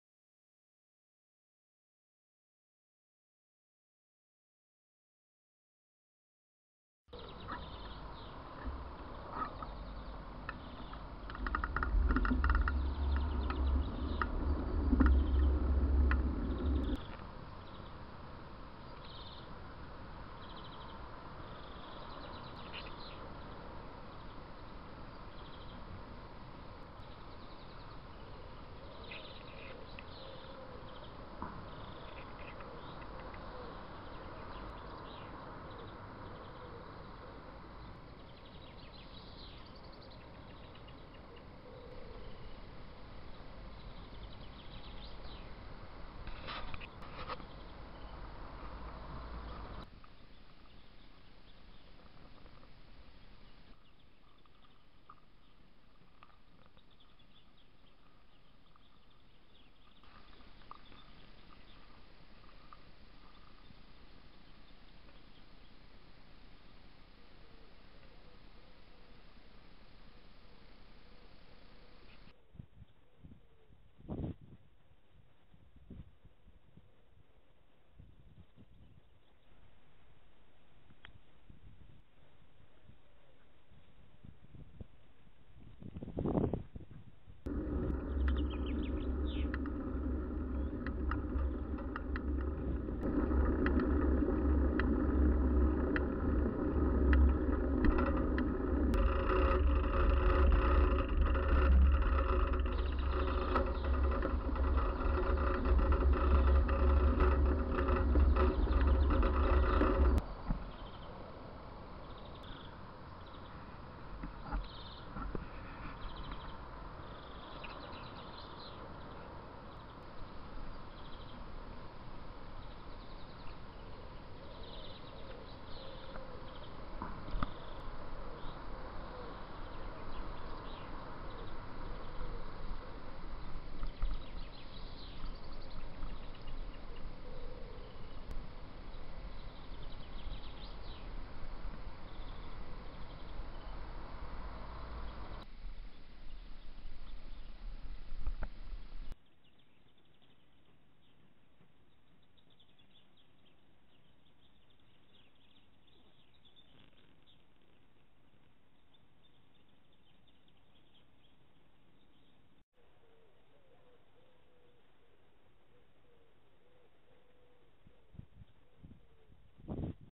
Sonntagsausflug
Ein kleiner Sonntagsausflug, durch den Hermannshof, den Exotenwald über Lützelsachsen zurück nach Weinheim.